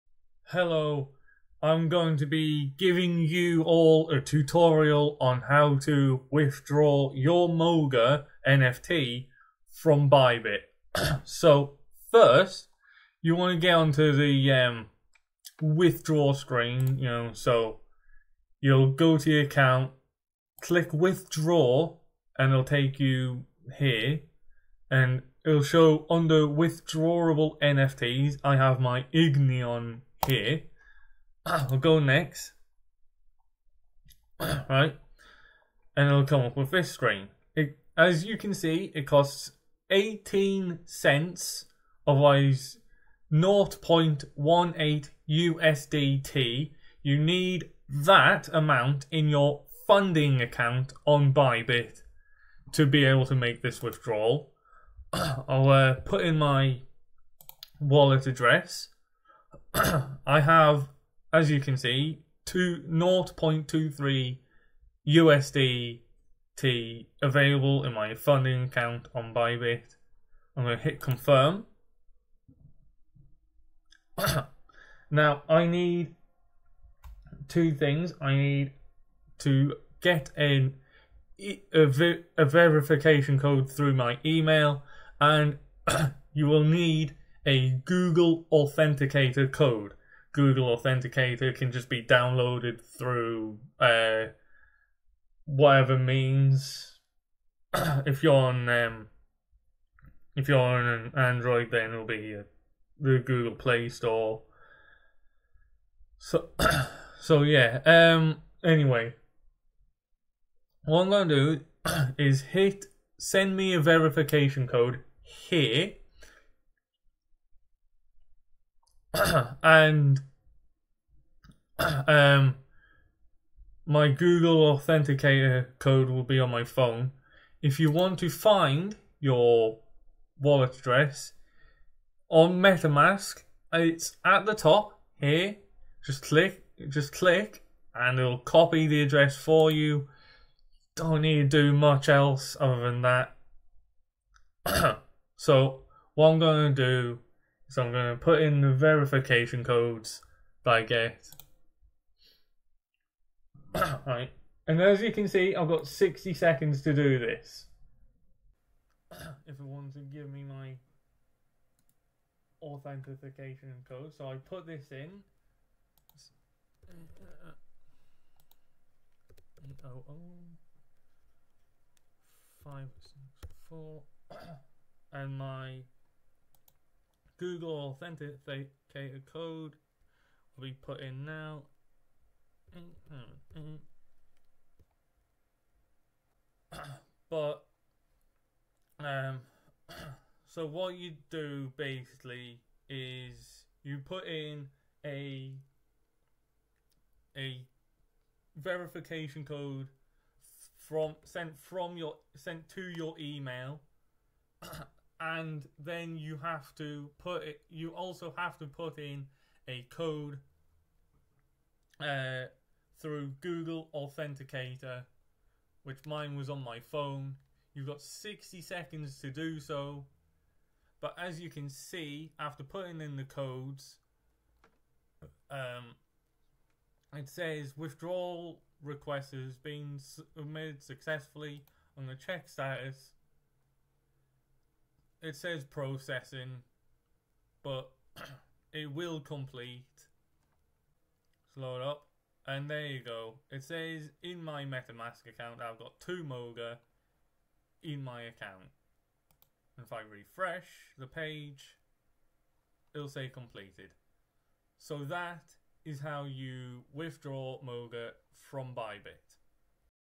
Tutorial on How To Withdraw Your Monster Galaxy Moga NFT from Bybit
1. Log in to Bybit.com and click on your profile picture. You’ll find it in the top right-hand corner of the page next to “Collections.”
2. Select “Withdraw” from the drop-down menu and you’ll be redirected to the Withdraw NFT page.
3. Choose one of your withdrawable NFTs and click “Next.”
4. Enter a Withdrawal Address and click the “Confirm” button. Make sure you have enough in your Funding Account to cover the Withdrawal Fee.
5. A window will pop up and you’ll need to click “Confirm” again to continue.
6. A Two-Factor Authentication pop-up will appear. Click on “Send me a verification code” and then check your email to get the code.
7. Enter the verification code you received via email as well as a Google Authenticator code. You will need to have the Google Authenticator App open to get the necessary code. Be aware that once you click on the “Enter email verification code” box, you will only have 60 seconds to input both codes and click the “Confirm” button.